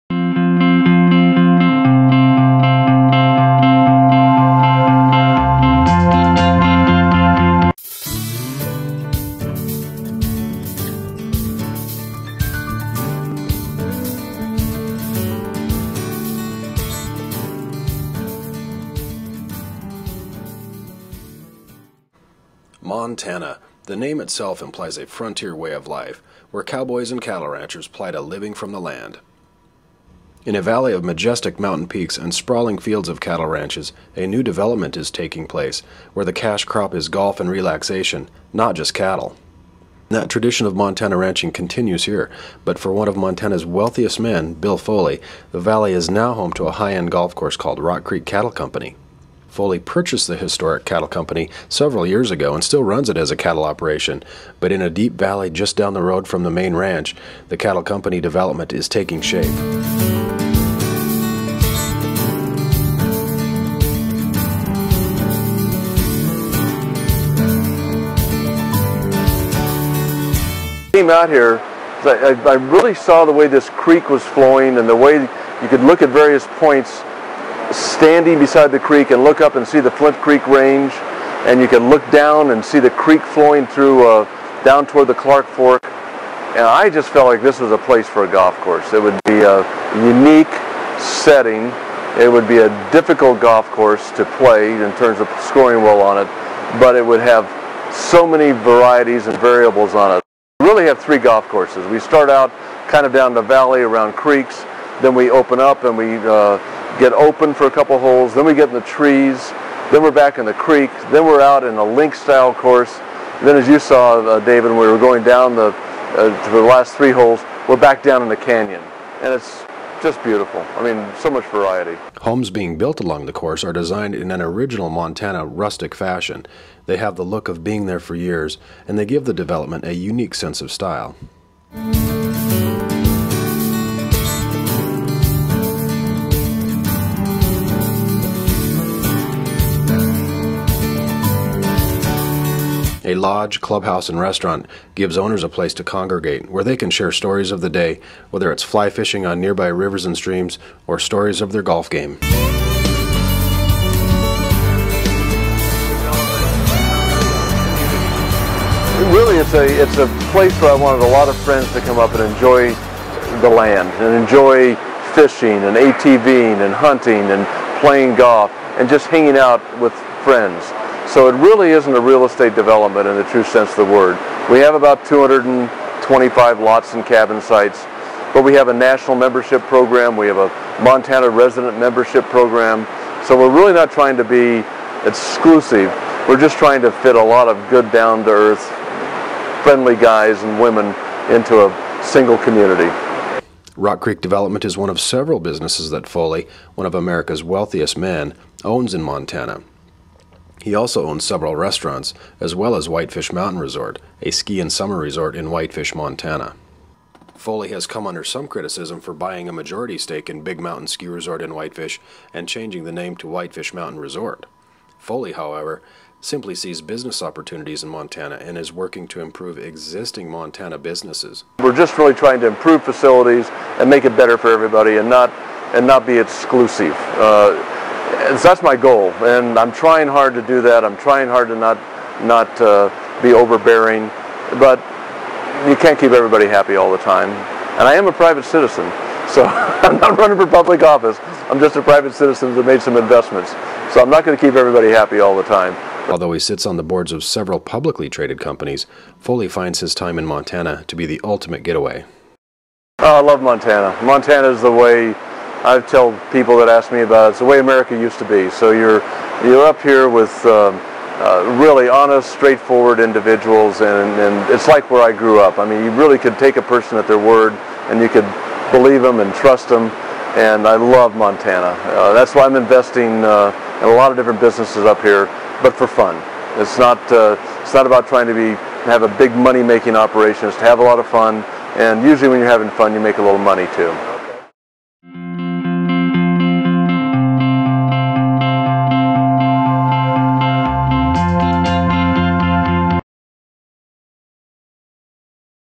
Rock Creek Cattle Co.
William P. Foley builds a high end golf course along side a historic Montana cattle Ranch in Deer Lodge